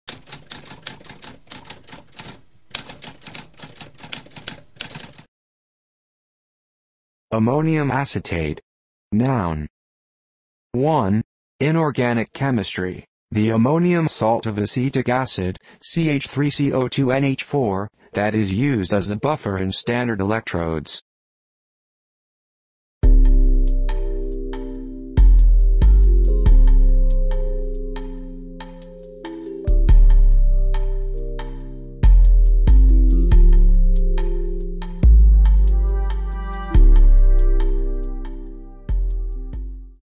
What does ammonium acetate mean?
A spoken definition of ammonium acetate.

Intro Sound:
Typewriter - Tamskp